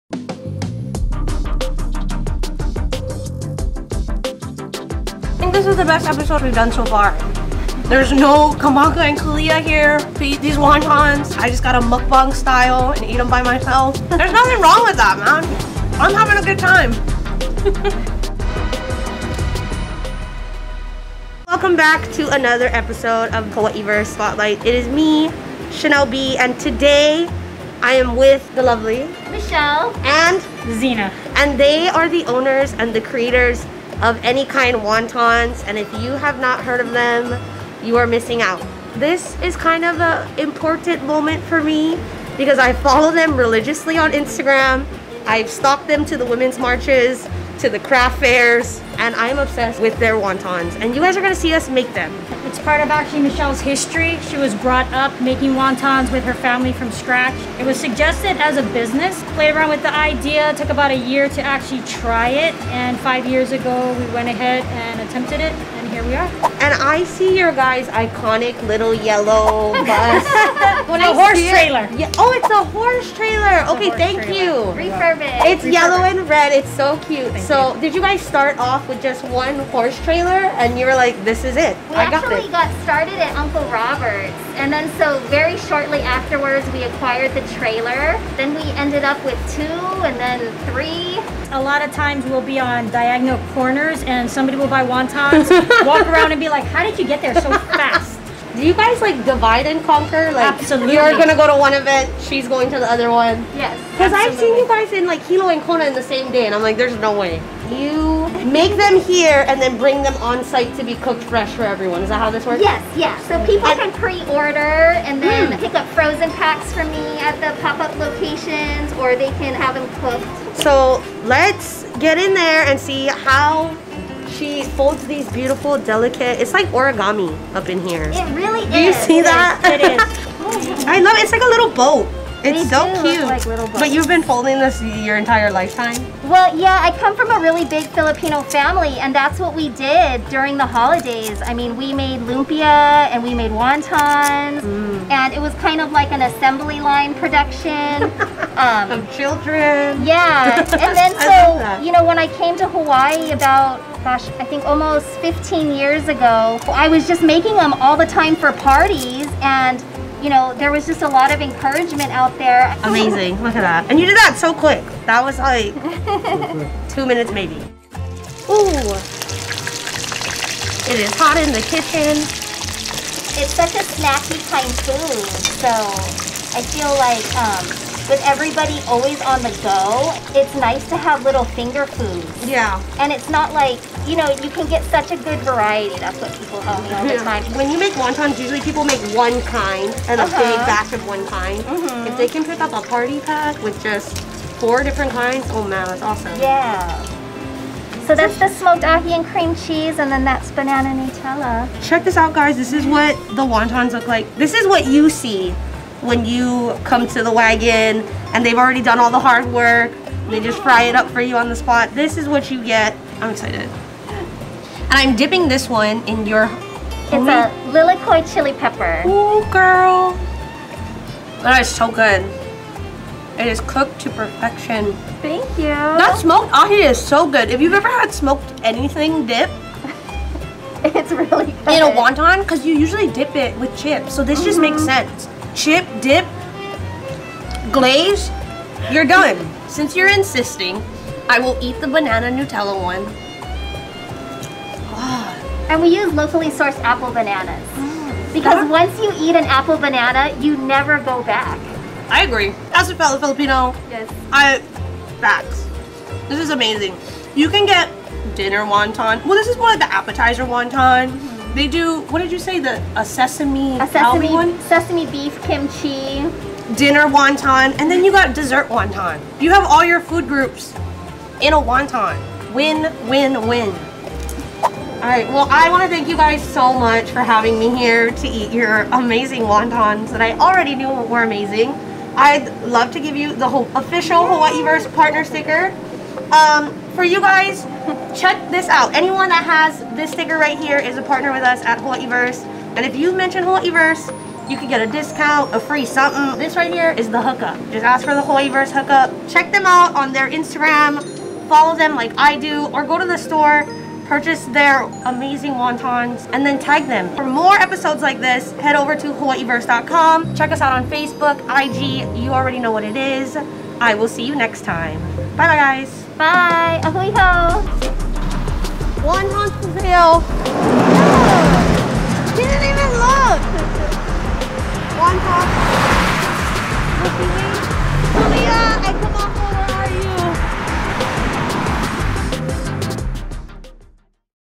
Hawaiiverse Spotlight: Any Kine Wontons | Hawaii Food Trucks
Any Kine Wontons is a Hawai’i Island, local business, that has taken a small idea and filled it (literally) with BIG flavors, from smoked ahi cream cheese to banana Nutella, they guarantee to have something to satisfy your deepest cravings! You can normally find their bright yellow food truck at local farmer’s markets & other places around town. They cater, deliver, stock selected local stores, & provide fundraising opportunities too!

Special thanks to our videography partners 🎥 Taylor Martin Media